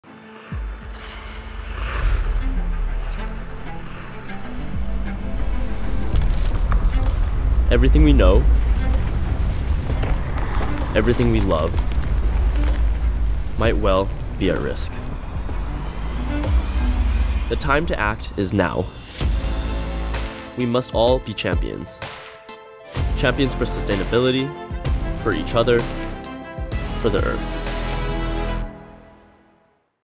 Panasonic GREEN IMPACT - Nathan Chen
Together, we can make a change. Alongside TeamPanasonic’s Nathan Chen, we’re addressing the climate crisis through our GREENIMPACT initiative to make our planet a more sustainable place.